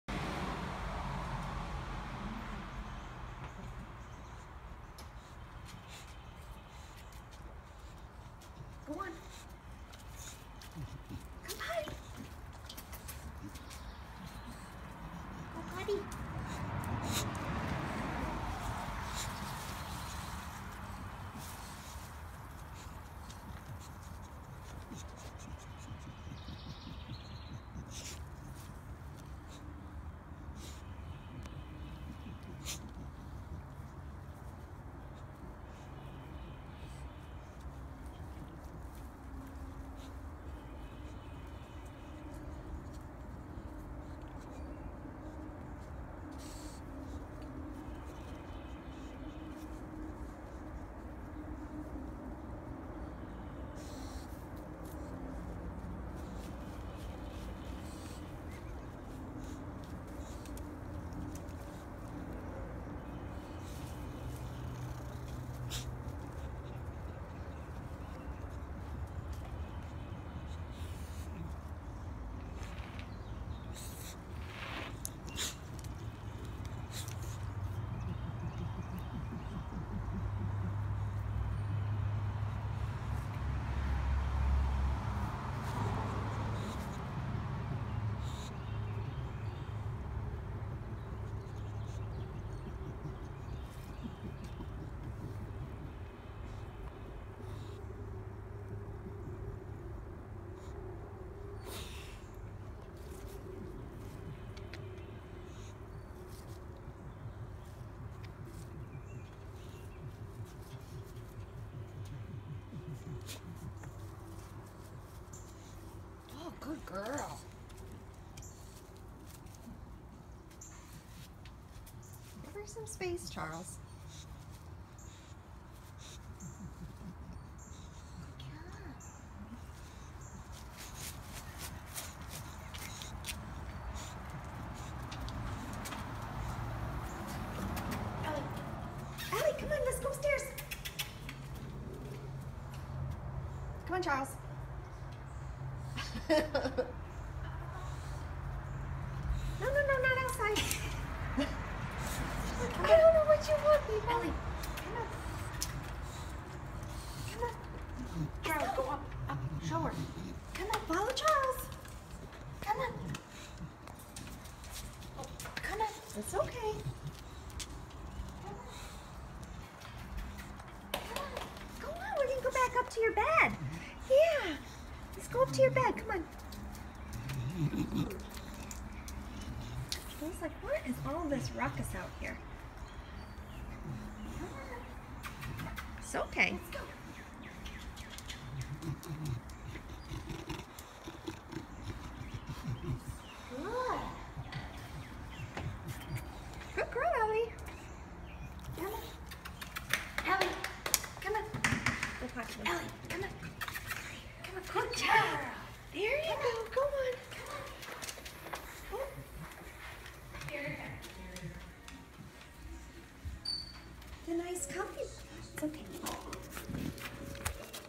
6-24-2017 Ellie outside
Meet Ellie.
This beautiful little girl was picked up as a stray in Indianapolis.
She has many wounds suggestive of being used as bait for dog fighting. Fresh wounds as well as old scarring. Her ear is badly injured and narcotic tissue was remove during her initial ER Visit, which has left the ear split open. Possible amputation may be in required. Many people have asked about the purple ink all over her body- often times bait dogs are covered in dye to more easily identify them. Because Ellie was found wondering in an abandoned industrial area, we are unsure her true history. Ellie was terrified of her own shadow when first found, but she's slowly opening up and trusting again.  Ellie will be available for adoption with Illinois English Bulldog Rescue once she is medically and emotionally ready.
If you would like to donate to her care, and click the Donate button